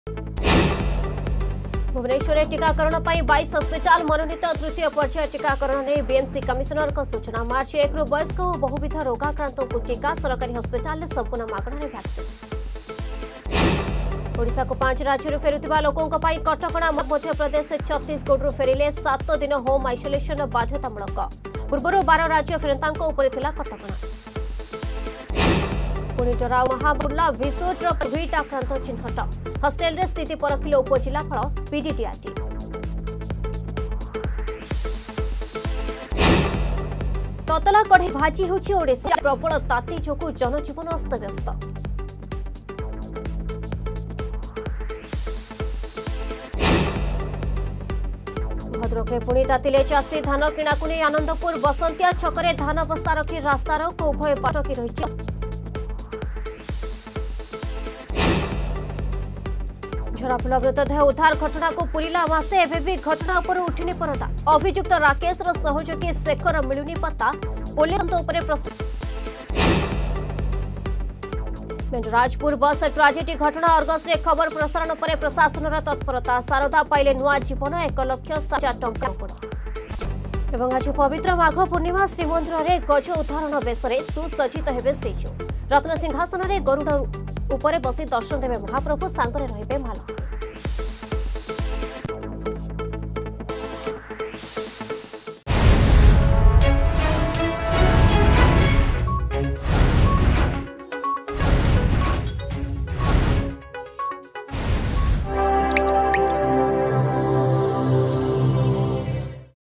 Headlines 2pm
ଭୁବନେଶ୍ୱରରେ ଟୀକାକରଣ ପାଇଁ 22 ହସ୍ପିଟାଲ ମନୋନୀତ...
ଓଡ଼ିଶାକୁ 5 ରାଜ୍ୟରୁ ଫେରୁଥିବା ଲୋକଙ୍କ ପାଇଁ କଟକଣା...
ପୁଣି ଡରାଉଛି ମହାମାରୀ କୋରୋନା...
ତତଲା କଡେଇରେ ଭାଜି ହେଉଛି ଓଡ଼ିଶା...
ଭଦ୍ରକରେ ପୁଣି ତାତିଲେ ଚାଷୀ...
ଝରାଫୁଲ ମୃତଦେହ ଉଦ୍ଧାର ଘଟଣାକୁ ପୁରିଲା ମାସେ...
ମେଣ୍ଡରାଜପୁର ବସ ଟ୍ରାଜେଡି ଘଟଣା...
ଆଜି ପବିତ୍ର ମାଘ ପୂର୍ଣ୍ଣିମା...